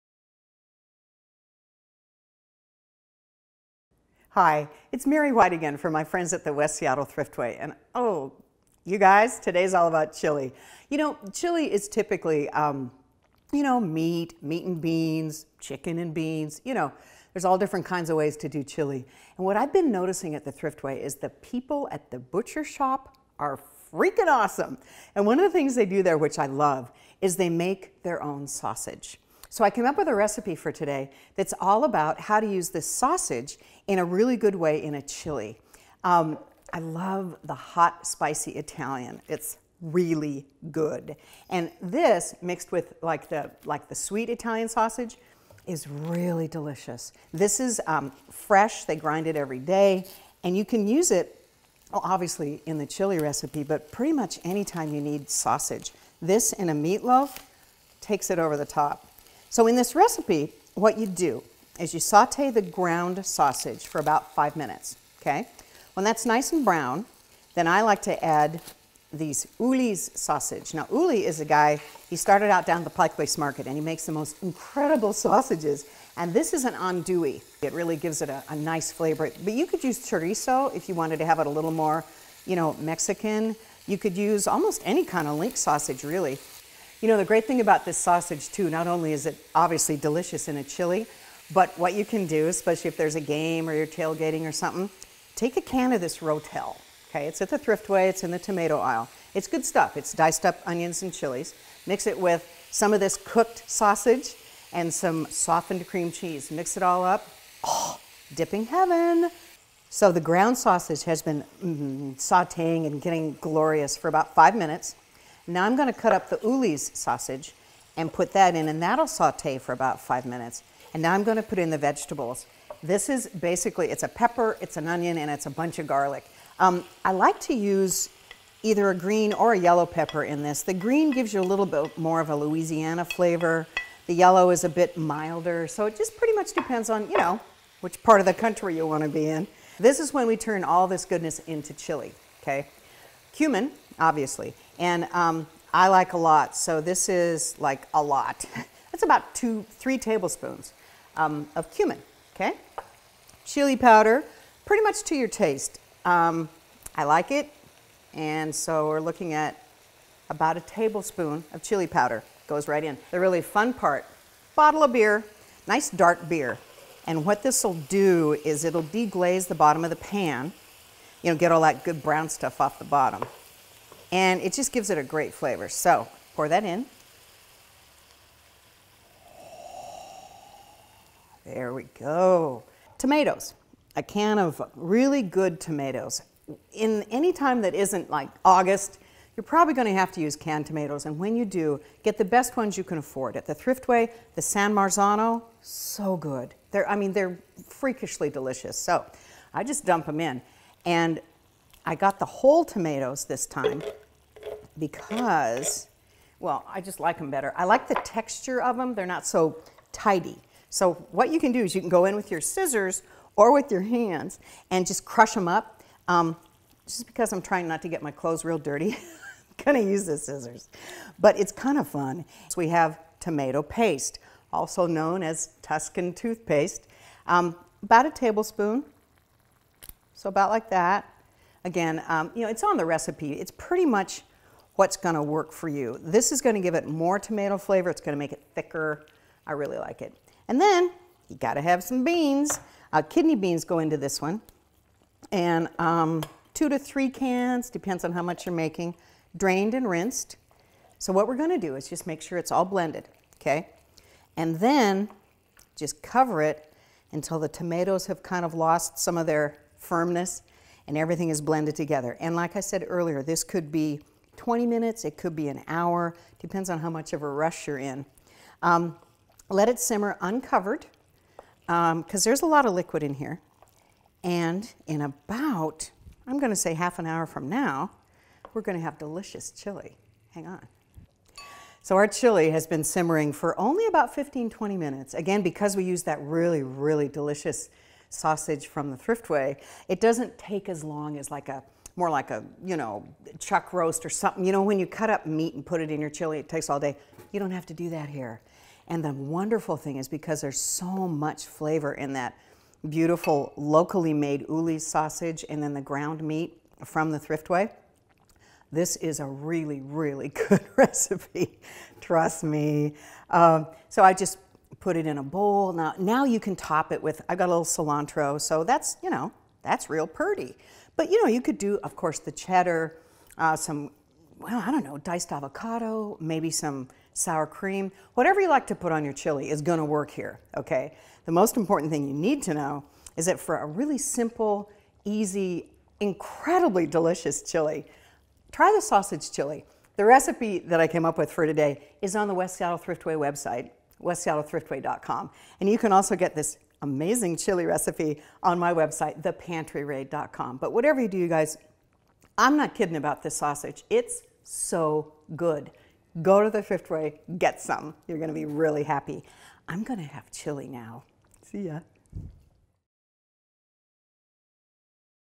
Easy Sausage Chili
Mary White has put together a great recipe for chili that won't take you all day. Check out this tasty treat, Spicy Sausage Chili.